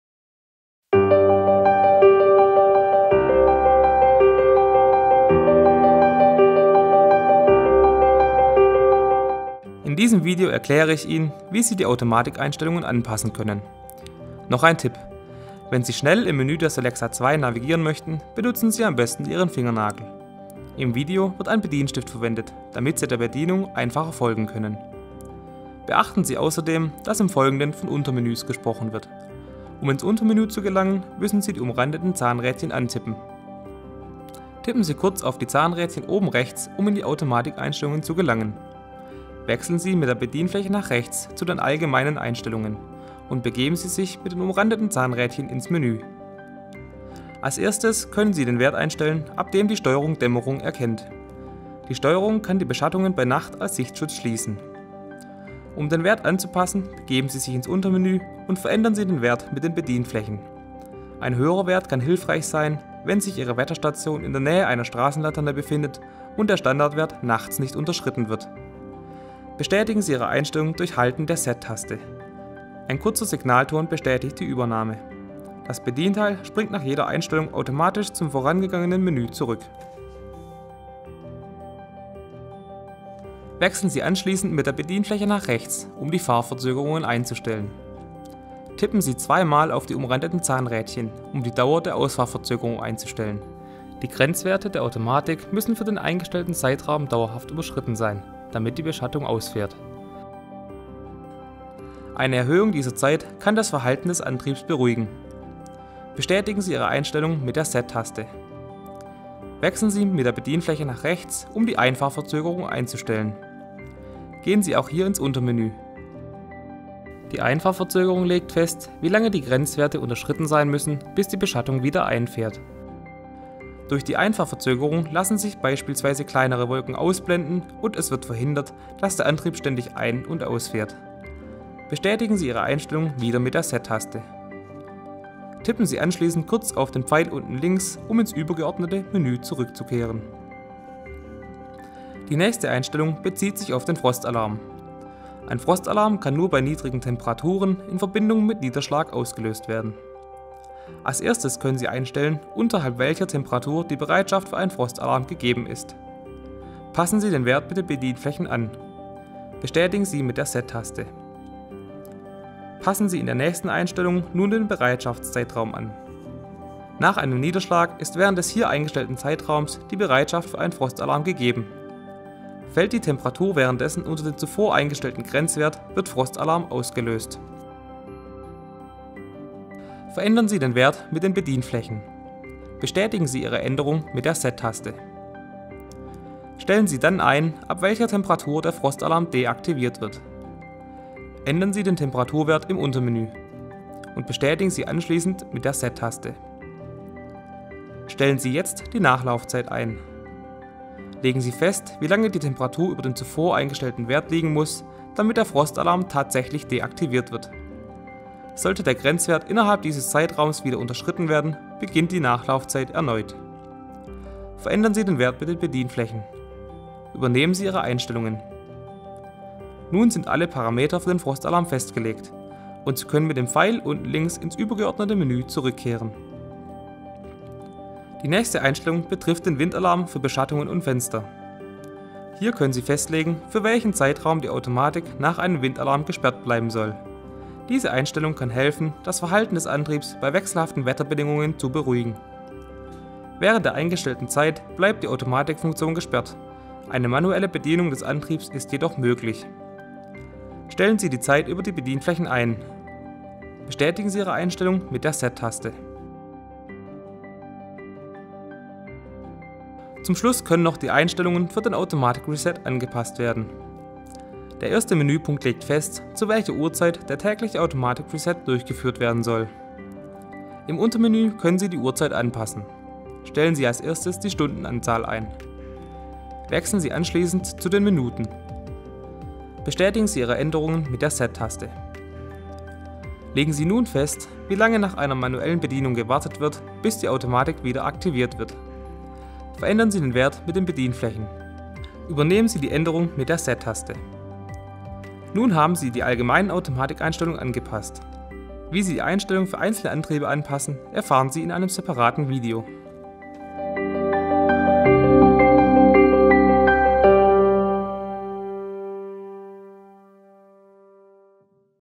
Solexa II – 06. Allgemeine Automatikeinstellungen
Dieses Video zeigt, wie die allgemeinen Automatikeinstellungen für das Steuerungssystem Solexa II angepasst werden können.

Untertitel auf Deutsch und Englisch verfügbar.